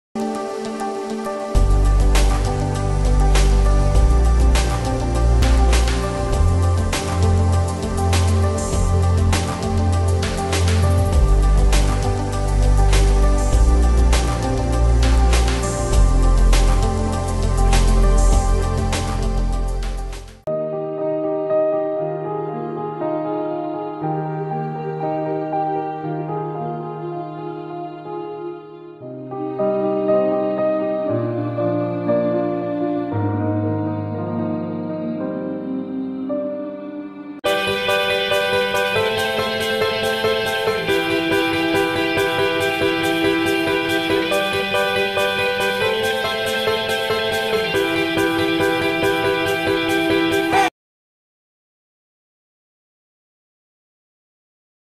Mentors Eduserve® Question Paper For Class~9¦ 2019-20
Hello Friends, In this video We are going to introduce a Mentors Question Paper of class-9 which are held in 2019-20 session.

Friends you can take Screen shots of Our Questions Paper From this video and try to make it.